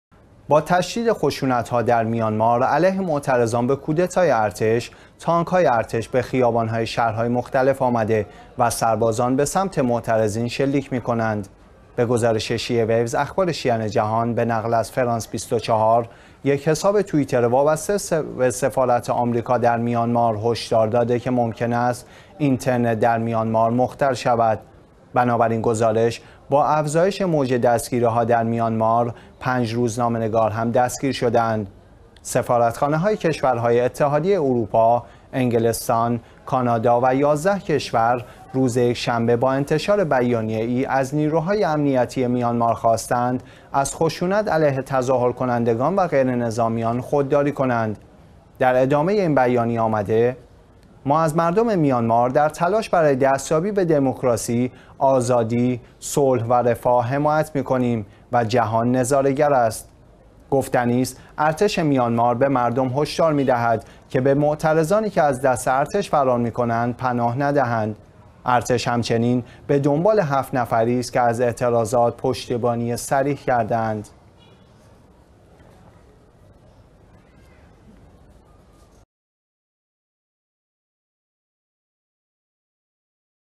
سرکوب گسترده و خشونت آمیز معترضان به کودتا در میانمار
با تشدید خشونت ها در میانمار علیه معترضان به کودتای ارتش، تانک های ارتش به خیابان های شهر های مختلف آمده و سربازان به سمت معترضین شلیک می کنند.
به گزارش شیعه ویوز (اخبار شیعیان جهان) به نقل از فرانسه ۲۴؛  یک حساب توئیتر وابسته به سفارت آمریکا در میانمار هشدار داده که ممکن است اینترنت در میانمار مختل شود.
بنا بر این گزارش، با افزایش موج دستگیری ها در میانمار پنج روزنامه نگار هم دستگیر شده اند.
سفارتخانه های کشورهای اتحادیه اروپا، انگلستان، کانادا و ۱۱ کشور روز یکشنبه با انتشار بیانیه ای از نیروهای امنیتی میانمار خواستند از خشونت علیه تظاهرکنندگان و غیرنظامیان خودداری کنند.
در ادامه این بیانیه آمده: ما از مردم میانمار در تلاش برای دستیابی به دموکراسی، آزادی، صلح و رفاه حمایت می کنیم و جهان نظاره گر است.
گفتنی است، ارتش میانمار به مردم هشدار می دهد که به معترضانی که از دست ارتش فرار می کنند پناه ندهند.
ارتش همچنین به دنبال هفت نفری است که از اعتراضات پشتیبانی صریح کرده اند.